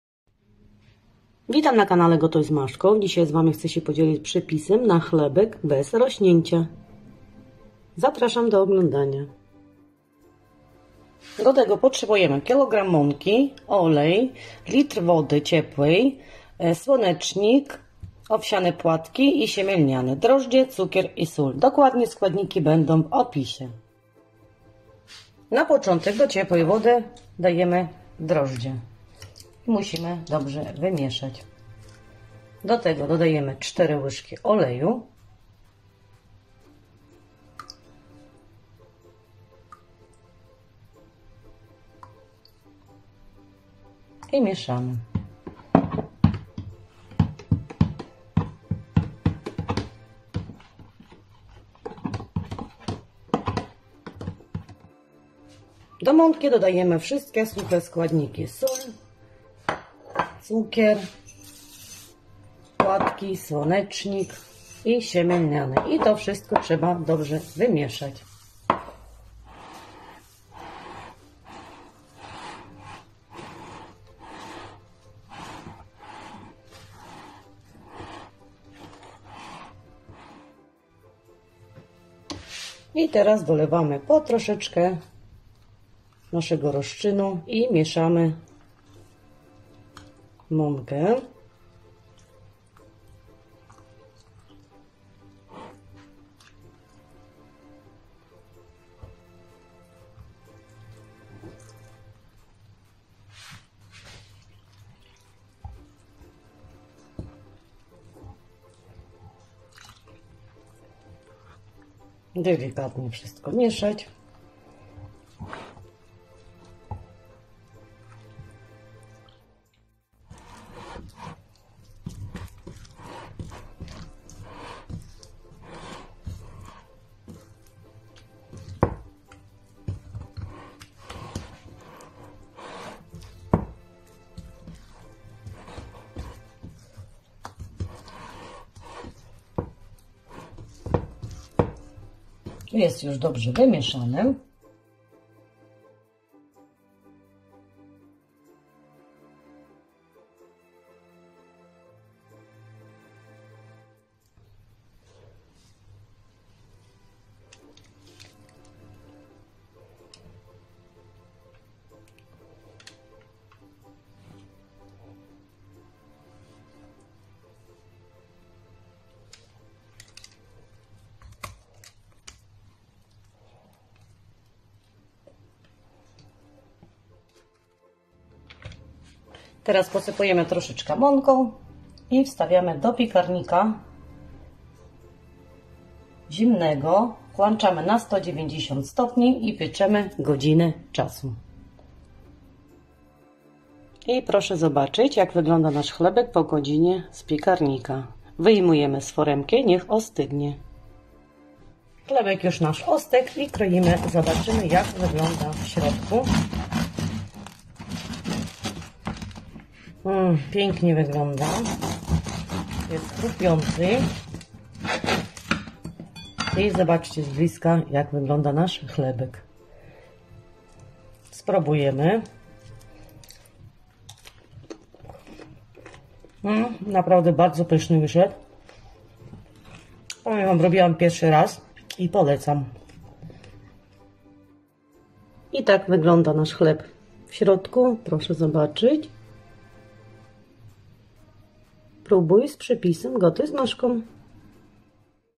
Chleb z ziarnami bez rośnięcia 😊/szybko i pysznie super przepis. Bread with grains without rising
Robiłam pierwszy raz ten chlebek i bardzo polecam jest bardzo prosty przepis.
Składniki :
1 litr ciepłej wody
10 dag drożdży
4 łyżki oleju
1 kg mąki
1,5 łyżki soli
0,5 łyżki cukru
1 szkl. płatek owsianych
1 szkl. słonecznika
0,5 szkl. siemię lniane
Można robić jeden duży chleb lub dwa mniejsze, zależy jakie macie blachy do pieczenia.
Wstawiamy w zimny piekarnik włączamy temperaturę na 190 C z termoobiegiem i pieczemy 1 godzinę.
Bardzo prosty przepis koniecznie spróbuj zrobić 😊
Zapraszam na moje ebooki
1.Gotuj z Maszką

2. Od śniadania do kolacji
I made this bread for the first time and I highly recommend a very simple recipe.
Components :
1 liter of warm water
10 grams of yeast
4 tablespoons of oil
1 kg of flour
1.5 tablespoons of salt
0.5 tablespoons of sugar
1 cup oatmeal
1 cup sunflower seeds
0.5 cups linseed
You can make one large bread or two smaller ones depending on the baking trays you have.
Put in a cold oven, turn on the temperature to 190 C with hot air and bake for 1 hour.
A very simple recipe, be sure to try to make 😊
1.Cook with Masha

2. From breakfast to dinner